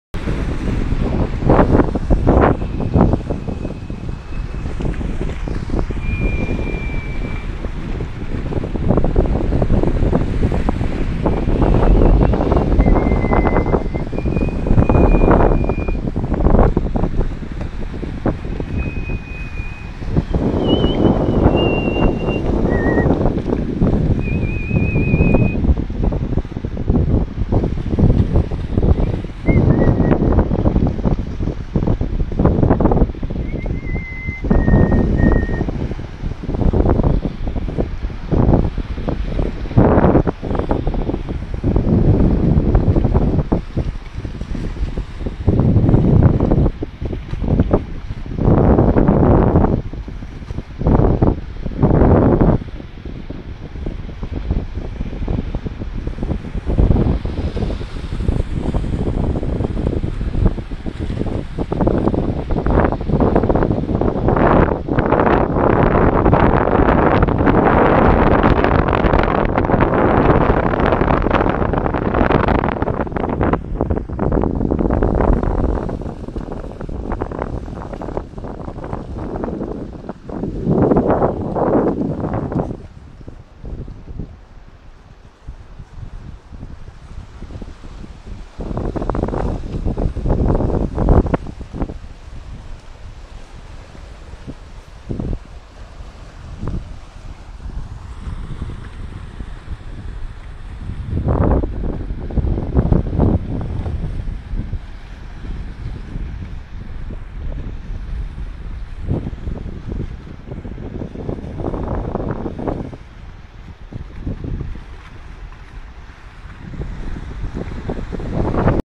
HENjUS TV ,... 2015 Memory ,... Hawaii - Lanikai Beach ,. East Oahu , 25 Miles from Waikiki ,..
The Traveling ''TENOR'' ,.. Lanikai Beach ,... is 9 Hours walking distance ,. one way ,. from Waikiki ...